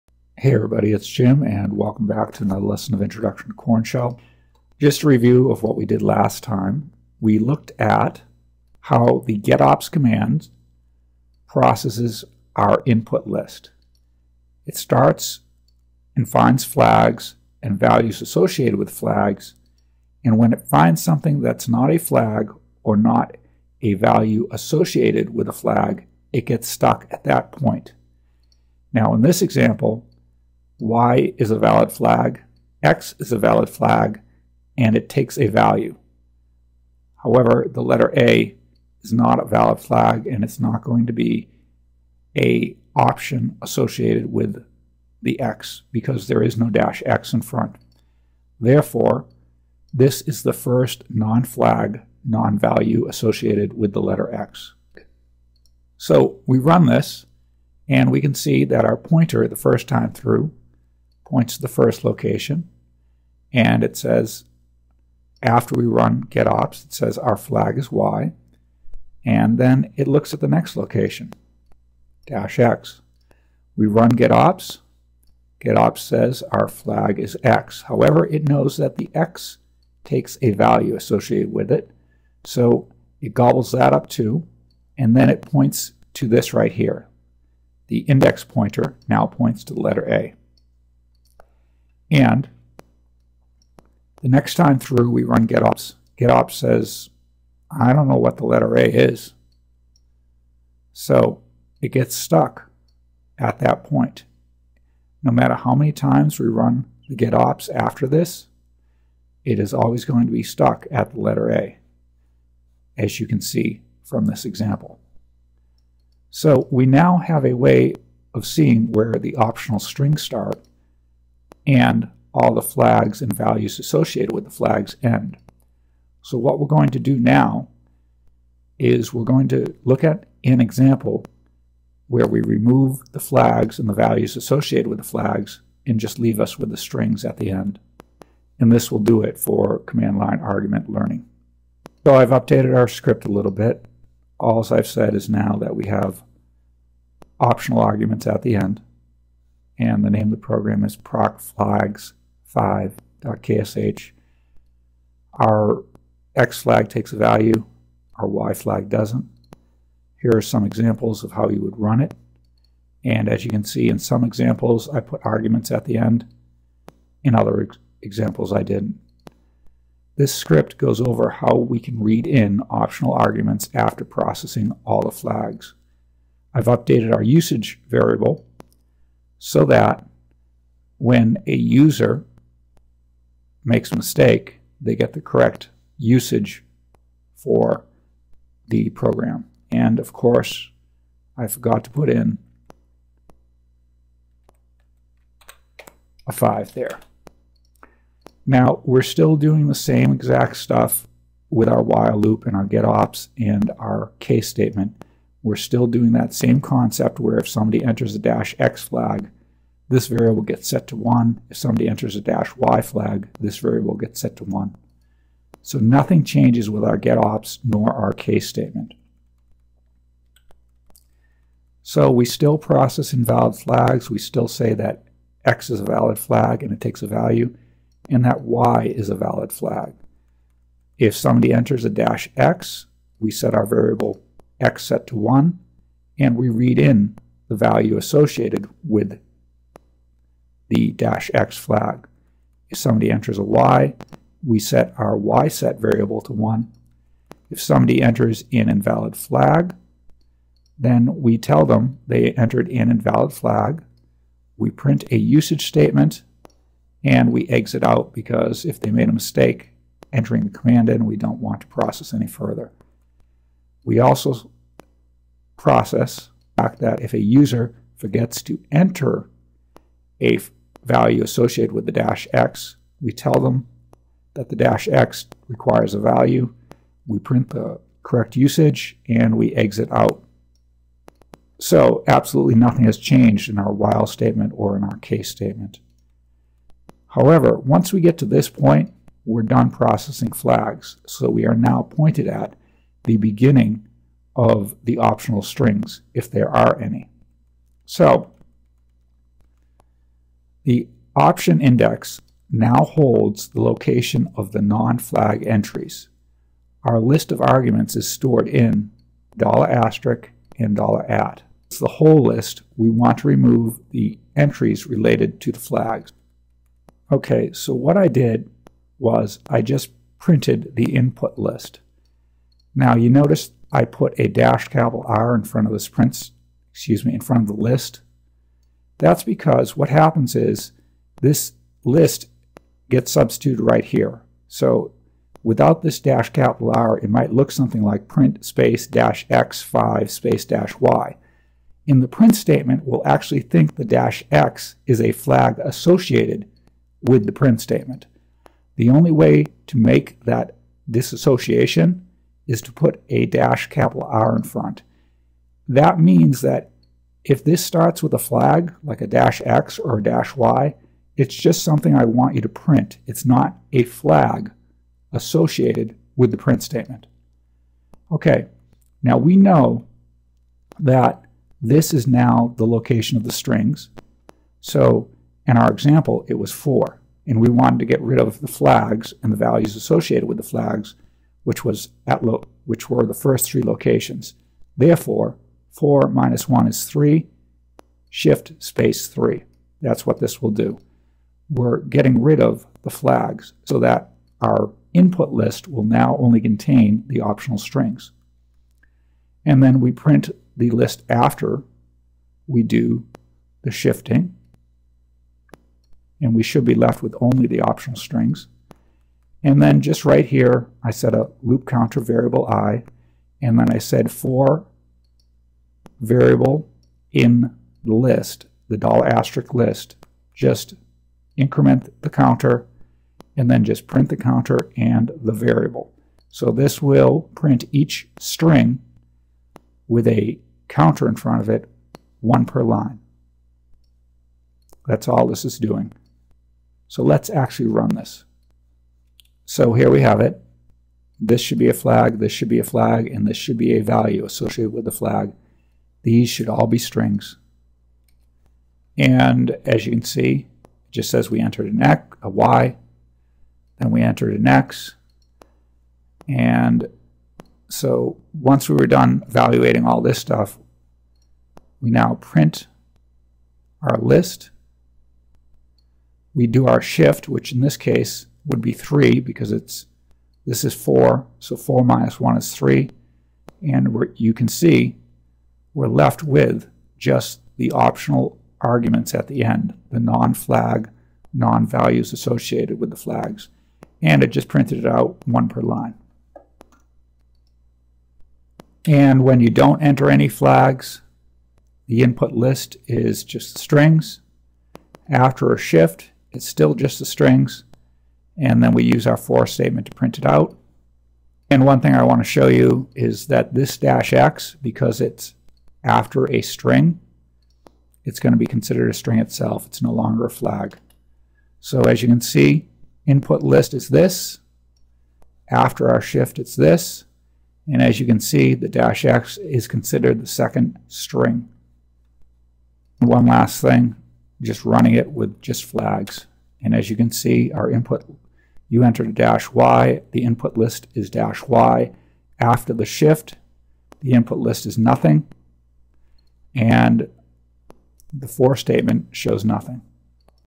Intro To Korn Shell - 15g Processing args after processing flags
Shows how you can use the pointer OPTIND to remove
flags from the input argument list after you are done processing
the flags.
(( X = OPTIND - 1 ))
shift X